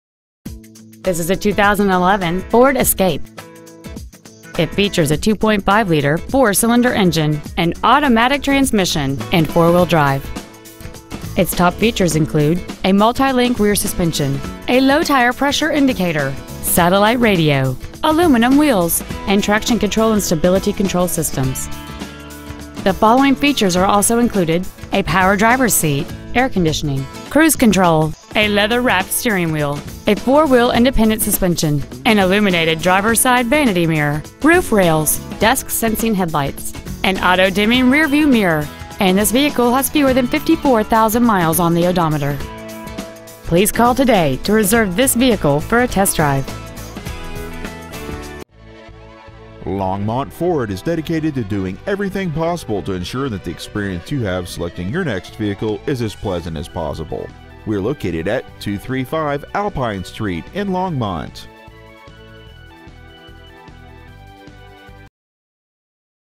2011 Ford Escape Longmont CO
Year : 2011
 Make : Ford
 Model : Escape
 Engine : 2.5L I4 16V MPFI DOHC
 Trans . : 6-SPEED AUTOMATIC
 Exterior : INGOT SILVER METALLIC
 Miles : 53,397
 Interior : CHARCOAL BLACK
 Stock : B17022